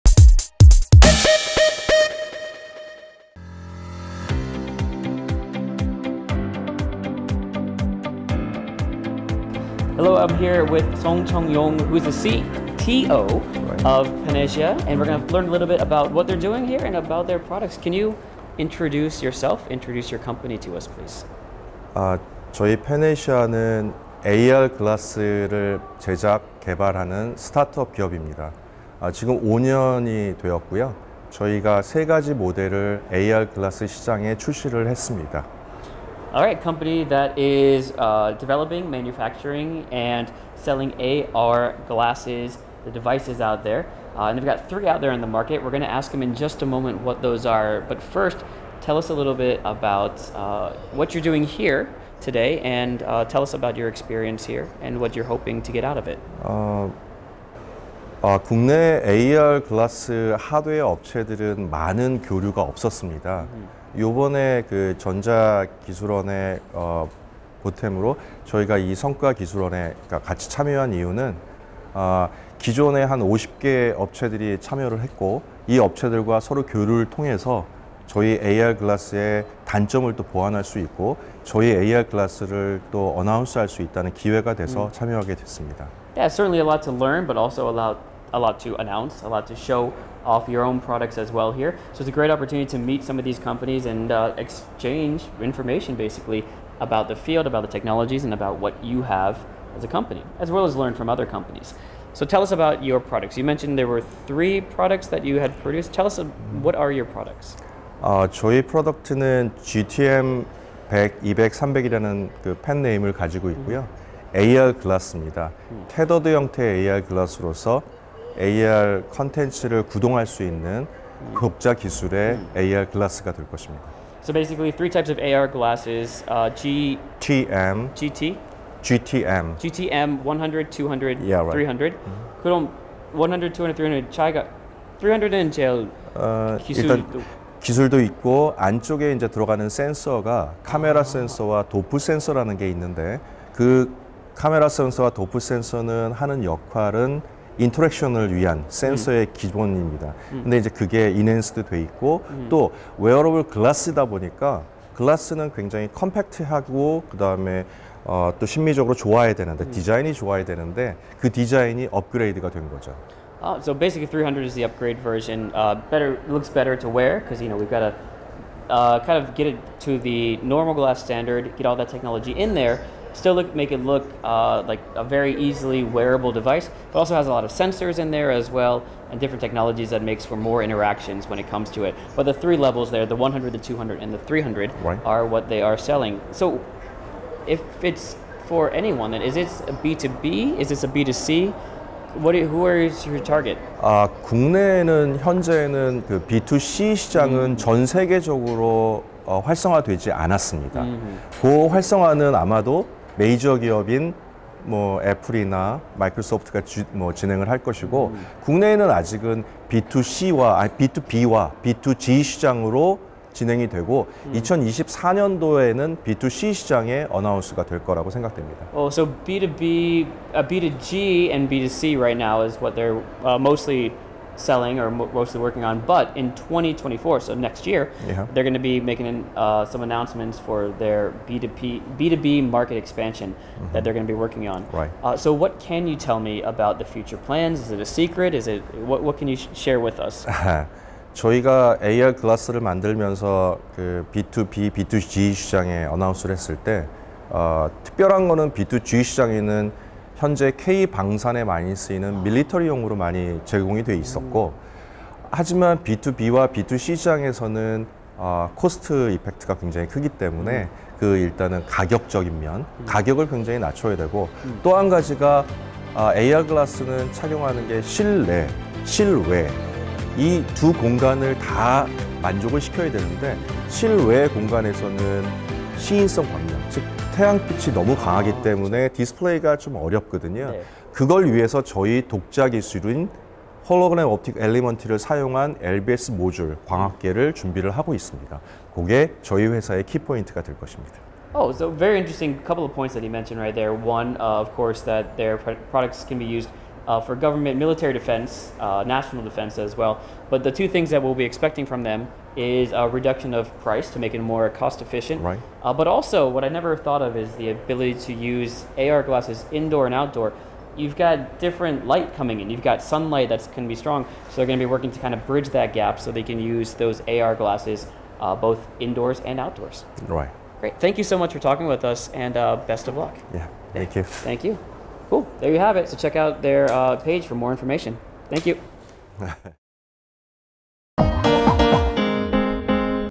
국내 XR 산업 생태계 활성화 위한 ‘2023년 XR 디바이스 산업 성과 교류회’ | 페네시아 인터뷰
정보통신산업진흥원(NIPA)이 전담하고 구미전자정보기술원(GERI) 주관 및 차세대융합콘텐츠산업협회(NCIA)가 운영한 ‘2023년 XR 디바이스 산업 성과교류회’가 지난 17일(목) 코엑스 스튜디오 159에서 성황리에 진행됐다.

이번 성과교류회에는 구미전자정보기술원이 지난 5월 공고한 ‘2023년 컨소시엄형 XR 디바이스 완제품 제작 및 상용화 지원사업’에 선정된 5개의 컨소시엄(주관기업 5개 사, 참여기업 12개 사) 임원이 참여했다. 2023년 컨소시엄형 XR 디바이스 완제품 제작 및 상용화 지원사업은 XR 디바이스 완제품 국산화와 상용화를 위한 지원 사업으로 컨소시엄 당 최대 3억 원을 지원한다.

이날 현장에서는 과제 개발 성과발표와 향후 계획, 기업 간 네트워킹 등이 부대행사가 마련됐으며, XR 디바이스 생태계 활성화를 위해 본 사업의 지원을 받은 기업들의 개발 역량과 향후 성장 가능성을 가늠할 수 있는 자리였다.

구정식 구미전자정보기술원 센터장은 “이번 컨소시엄형 XR 디바이스 완제품 제작 및 상용화 지원 사업에 매우 많은 기업이 지원했고, 아쉽게 탈락한 곳도 많다. 내년에는 이 자리에서 많은 기업이 함께 했으면 한다”라며 “제1회 성과교류회인 만큼 여기에 모인 기업을 중심으로 많은 참여 기관 관계자들이 XR이라고 하는 공간의 컴퓨팅 세계를 함께 열어줄 것이라고 기대한다”고 말했다.

이어 “특히 XR 산업 생태계를 육성하는 것이 제 역할이다. 그간 XR 산업 생태계 안에서 많은 기업이 헌신적으로 부품이나 소재 관련된 많은 일을 해왔다. 오늘이 시작인 만큼 내년에도 XR 산업이 크게 성장할 수 있도록 최선을 다하겠다.”고 전했다.

컨소시엄 주관기업인 액츠㈜, ㈜파노비젼, ㈜페네시아, ㈜피앤씨솔루션, ㈜홀로랩 등의 성과발표도 이어졌다.

송청용 페네시아 CTO는 “그동안 국내 AR 글라스 하드웨어 업체 간의 교류 기회는 많지 않았다. 관련 사업의 50여 개 기업이 참가한 이번 성과교류회를 통해 자사 기술을 알리고자 참여했다”라며, “AR 글라스는 실내외 두 공간을 만족해야 하지만 실외에서는 태양 빛이 강해 디스플레이를 구현하기 어렵다. 자사의 독자 기술 ‘HOE’(Hologram Optic Elements)를 주력으로 개발해 AR 글라스의 단점을 보완하겠다.”라고 말했다.

이어서 본사업의 주관기관인 구미전자정보기술원 구정식 센터장과 기업 간 간담회를 통해 기술 개발 관련 애로사항과 건의가 논의됐다.

XR은 가상현실(VR)과 증강현실(AR), 혼합현실(MR)을 통칭하는 몰입형 기술로 일부 전문가들은 XR 기술의 집합체인 XR 디바이스가 스마트폰을 비롯해 모바일 기기를 대체할 것이라는 전망을 내놓고 있다. 최근 애플 비전프로, 메타 퀘스트3 등 글로벌 기업들의 XR 디바이스 출시 경쟁도 이어지고 있다.

특히 구미전자정보기술원이 지원하는 ‘2023년 컨소시엄형 XR 디바이스 완제품 제작 및 상용화 지원사업’에 선정된 기업들은 실질적인 지원 정책에 대해 매우 높은 만족도를 나타냈다.

한편, 이날 열린 XR 디바이스 산업 성과교류회에 이어 오는 11월 9일(목)에는 상암 누리꿈스퀘어 비즈니스타워 3층에서 XR 디바이스 산업 기술교류회가 개최되며, 성과교류회에서 발표한 5개 컨소시엄의 개발 제품을 전시할 계획이다.